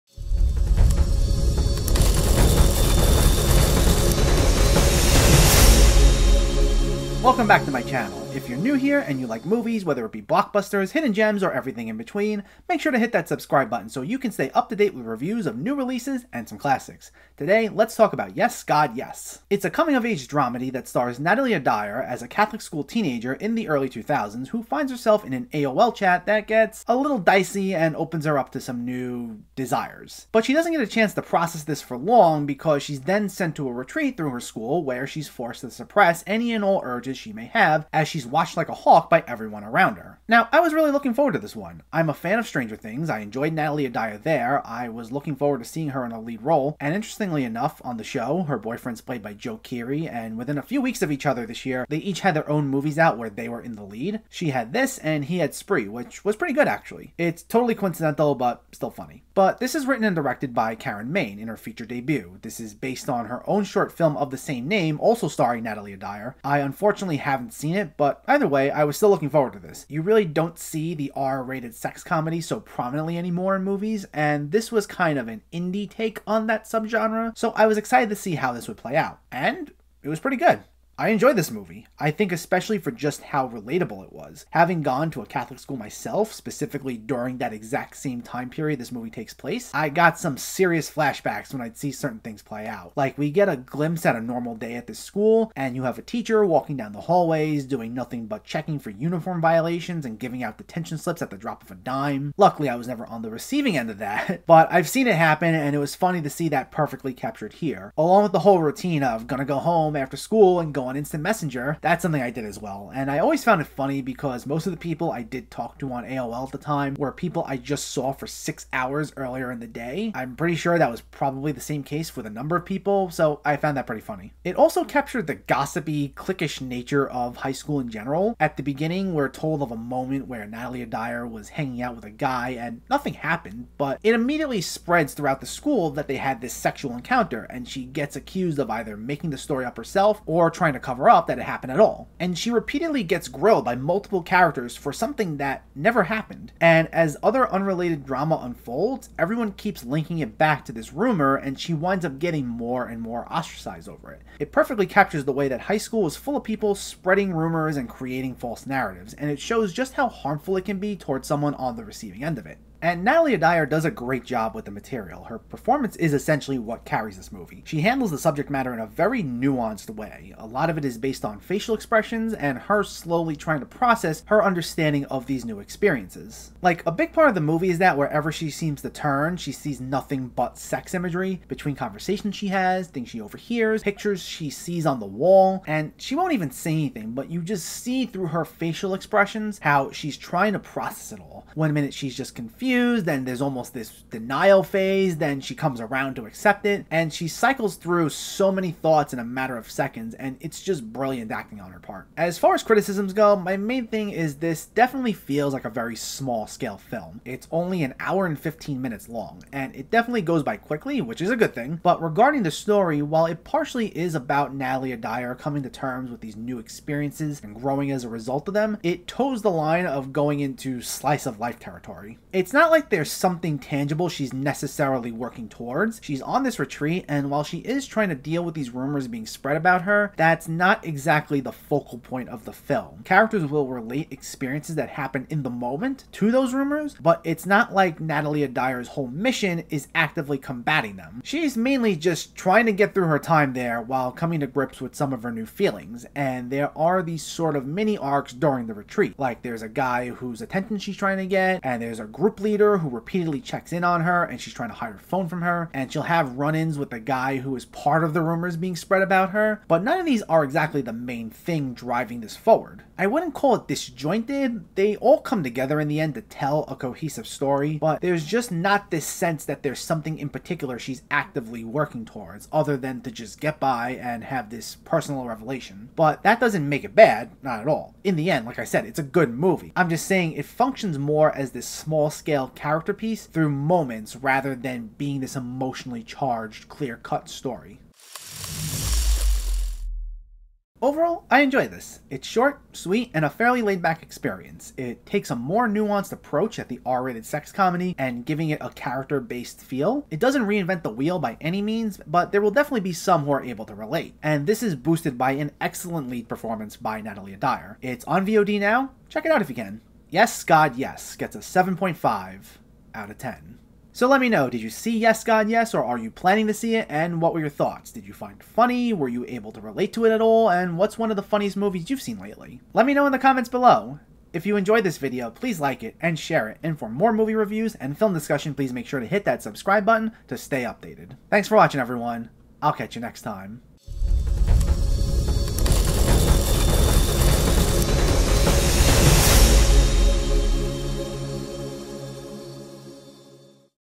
Yes, God, Yes (2020) - Movie Review | Why It's Good
Welcome to a new comedy movie review by Nick's Film Corner. Today I'm doing a Yes, God, Yes movie review, discussing why Yes, God, Yes is good, so you know all the details before you check it out.

Yes, God, Yes is a coming of age dramedy that stars Natalia Dyer as a Catholic school teenager in the early 2000s who finds herself in an AOL chat that gets a little dicy and opens her up to new desires. But she doesn’t get a chance to take this in for long because she’s then sent to a retreat through her school, where she’s forced to suppress any and all urges she may have, as she’s watched like a hawk by everyone around her.